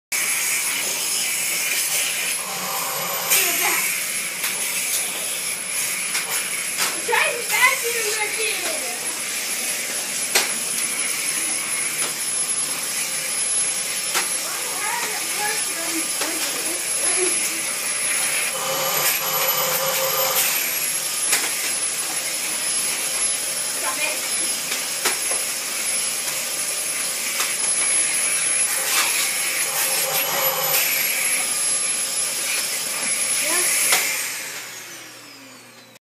Dog chases vacuum
Boxer puppy loves to chase the vacuum.